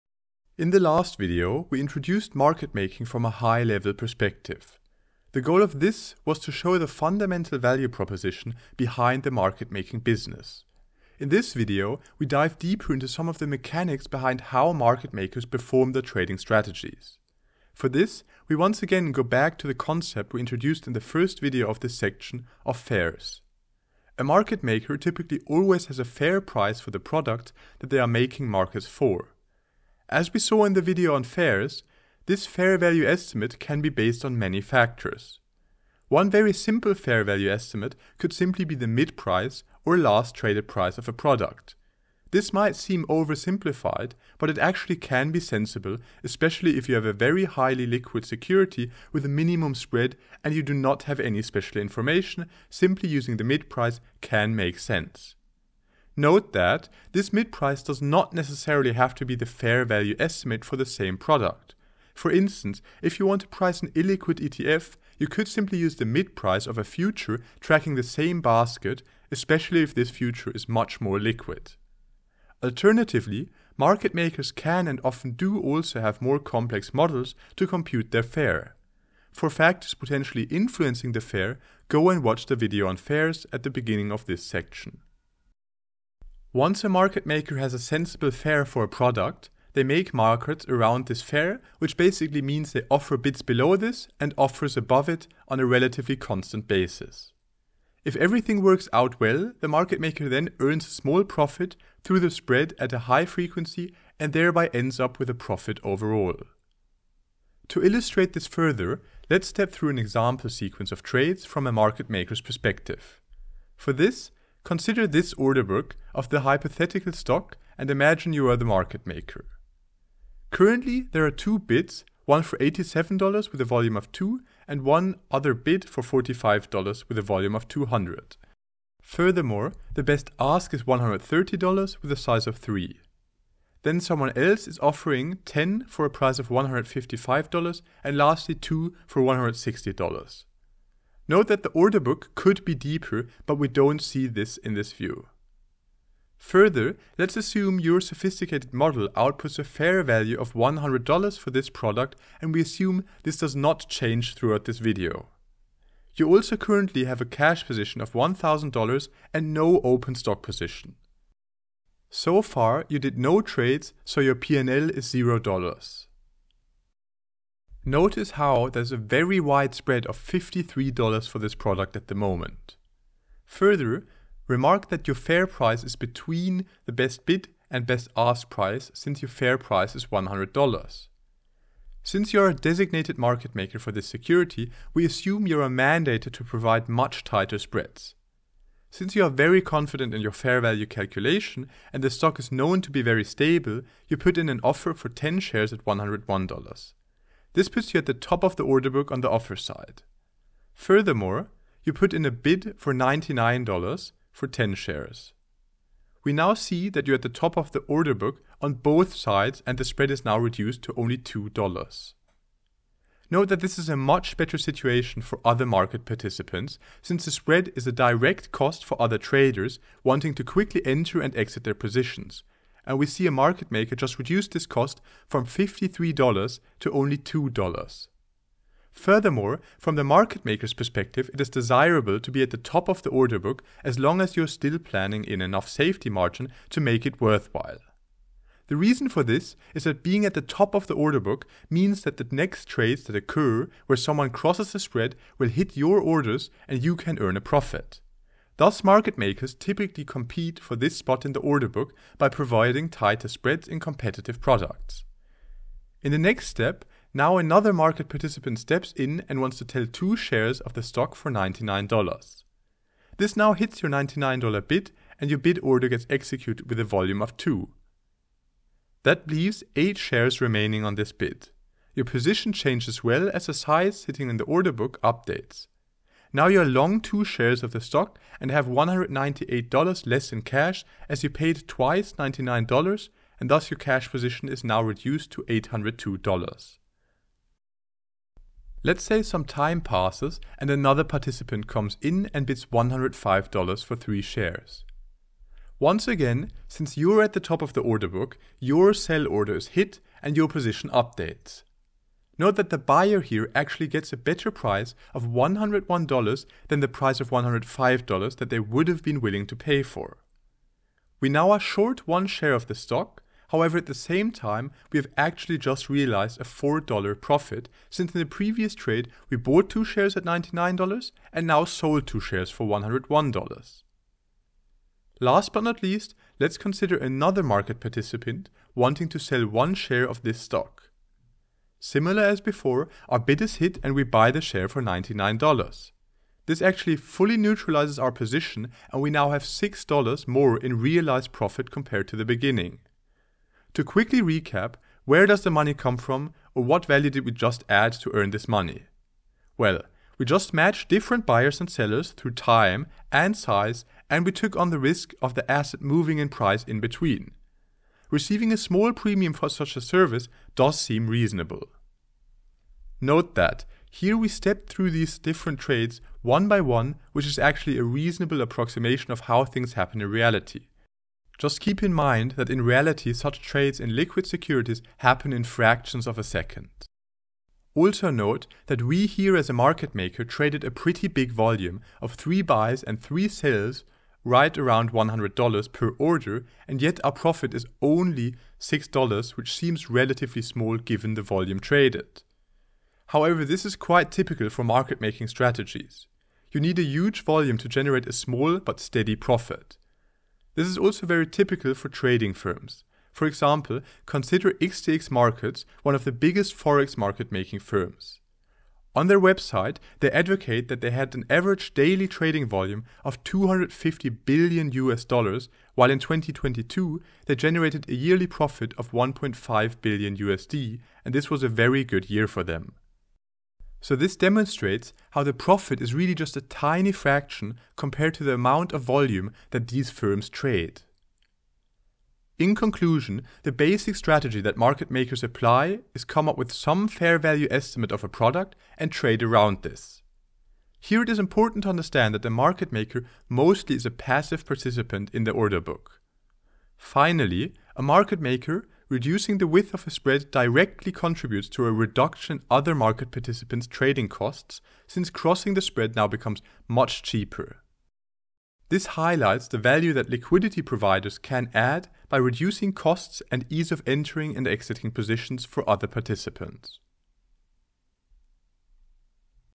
Order Book Strategies - Quant Trading Series
Check out the full course on how to crack a quant trading interview

This video is part of a mini-series where we cover all the details you need to know to understand market making which is highly relevant for most quant trading firms. Check out the remainder of the free series